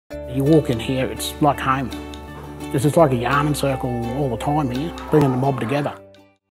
Kindy is like home
Elders and community leaders in Toowoomba and Cunnamulla are yarning about kindy and the importance of culture in the early years. Read more of their stories on The Early Years Count